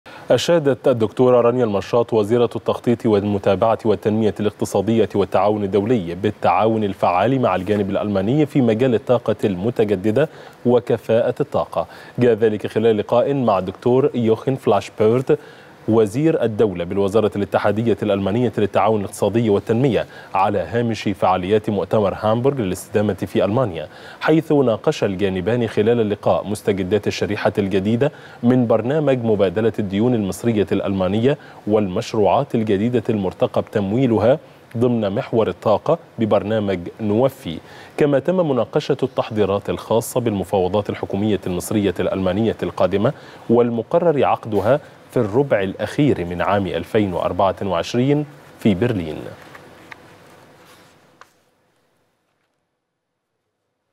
المشاط تبحث مع نظيرها الألماني جولة المفاوضات الثنائية لوضع أولويات التعاون خلال 2024 - 2026
وزيرة التخطيط والتعاون الدولي تبحث مع نظيرها الألماني استعدادات جولة المفاوضات الثنائية لوضع أولويات التعاون خلال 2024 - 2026